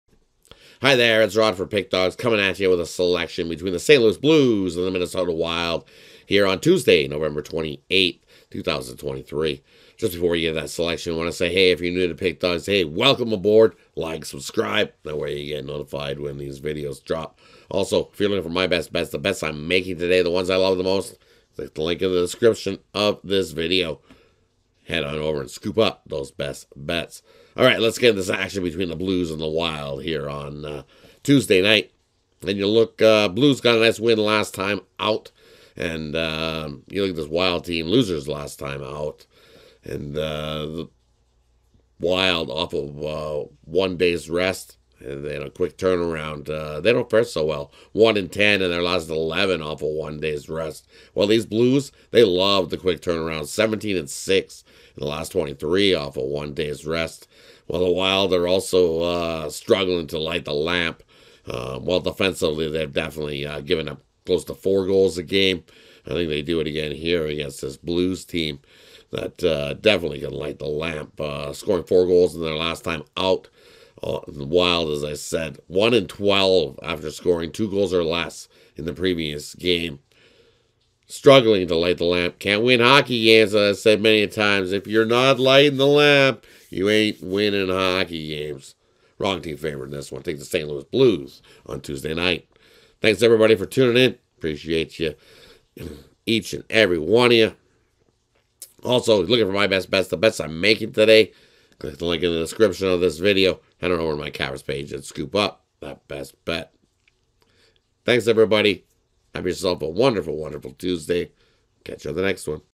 St. Louis Blues vs Minnesota Wild 11/28/23 NHL Free Pick | NHL Betting Tips
The Minnesota Wild face the St. Louis Blues in Tuesday's NHL action. Get Rodd's NHL Pick and Prediction for this game.